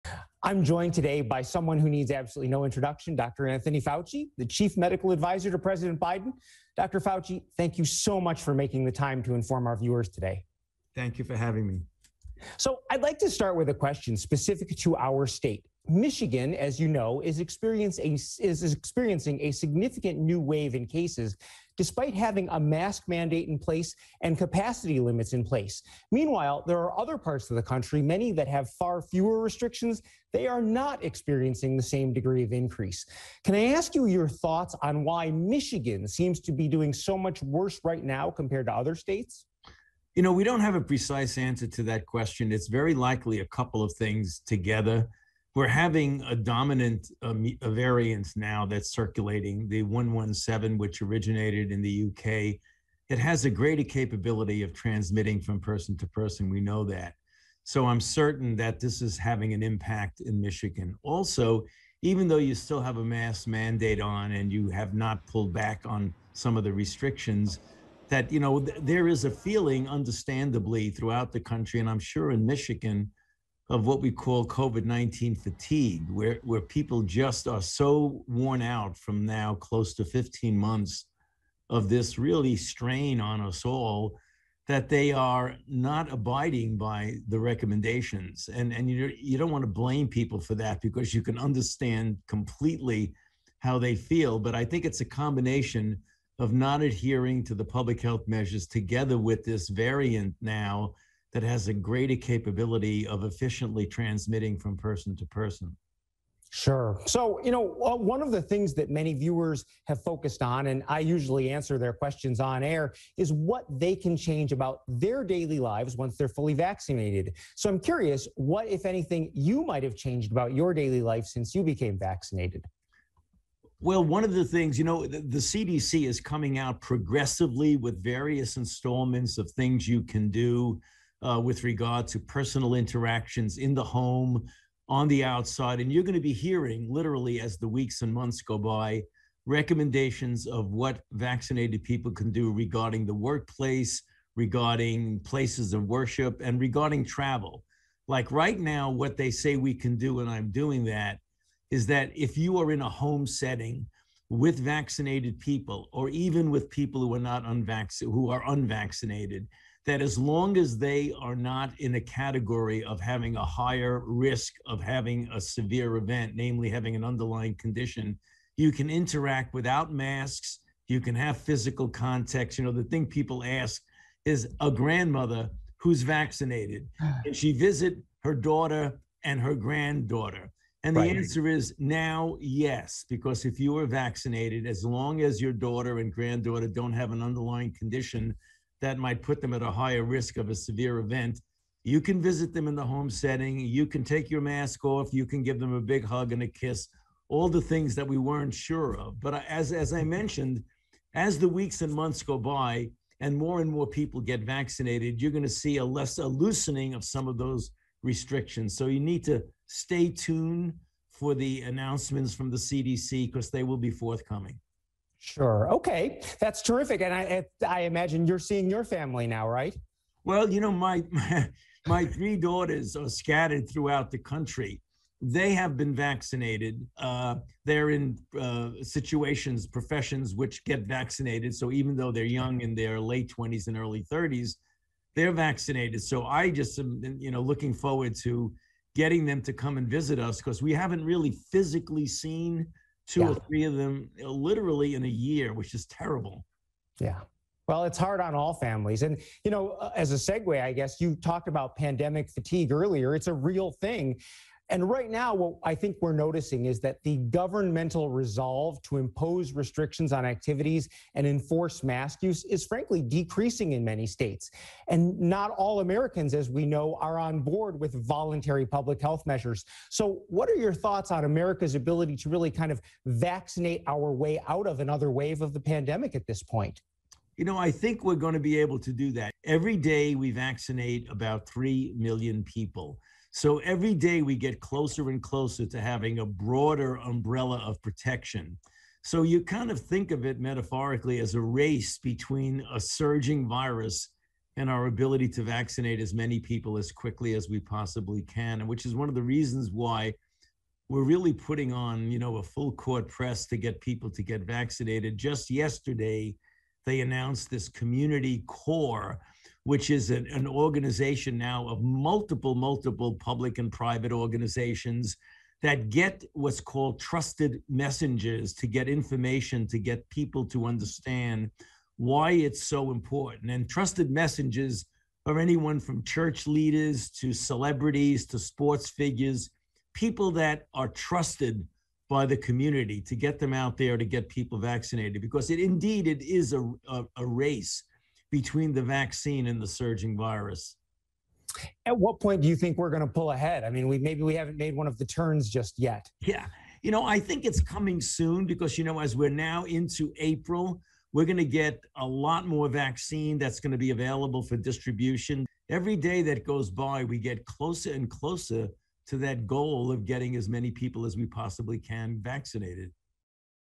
WDIV's Dr. Frank McGeorge sits down with Dr. Anthony Fauci part 1
WDIV's Dr. Frank McGeorge speaks with Dr. Anthony Fauci part 1